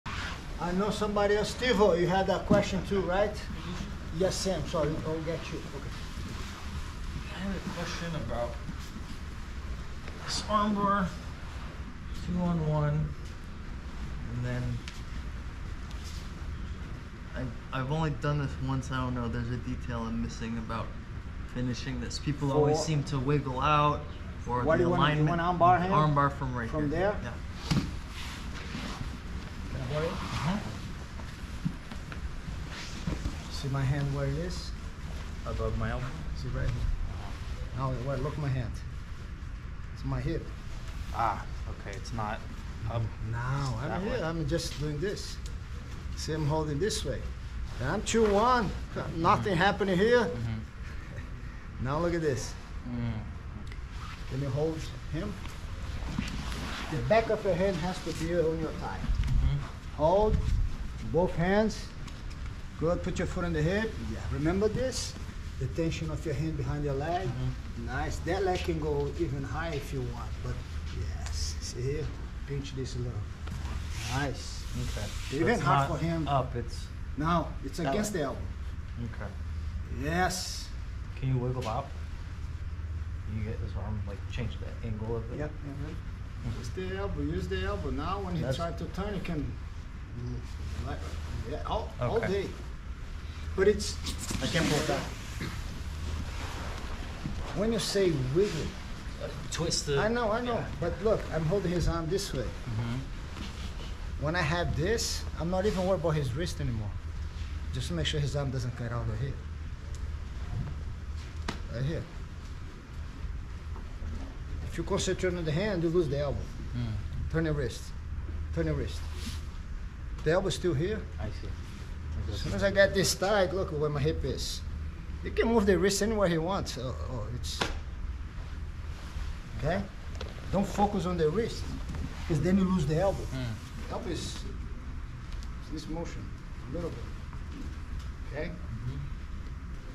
Minimalist Arm Bar
You can helicopter-flying-inverted-belly-down-360 arm bar all you want, but sometimes the short and sweet principle based arm bar will get you the sub you need.